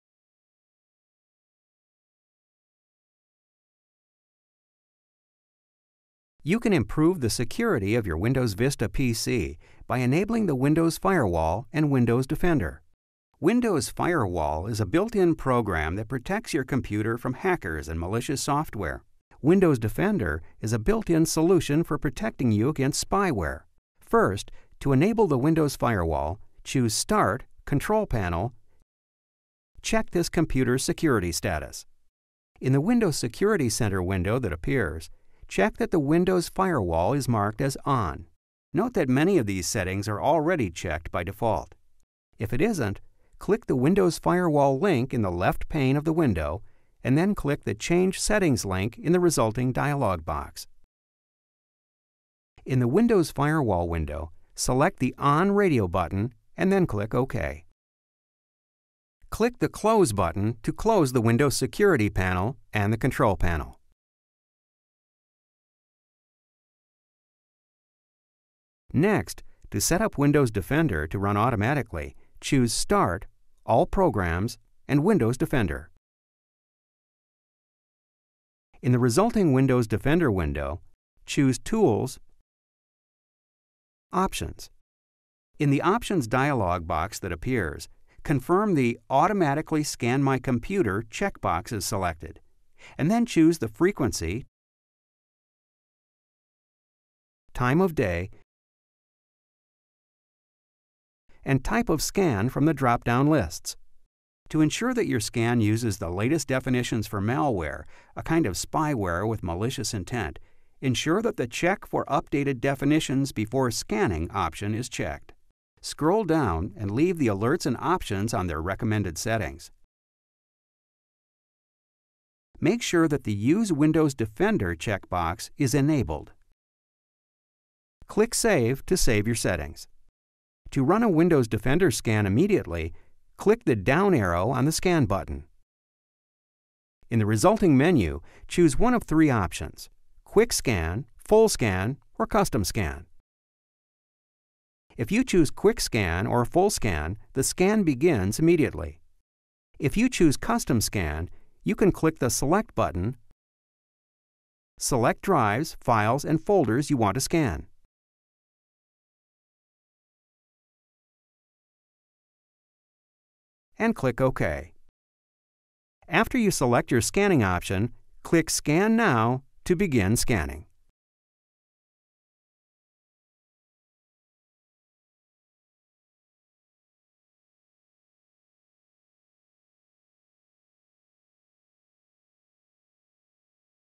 How to Enable the Windows Firewall and Windows Defender For Dummies
ou can easily increase Windows Vista security by turning on Windows Firewall and Windows Defender, which come free with Windows Vista. Enabling this free software can give you the relief of knowing that your Internet connection is secured and monitored.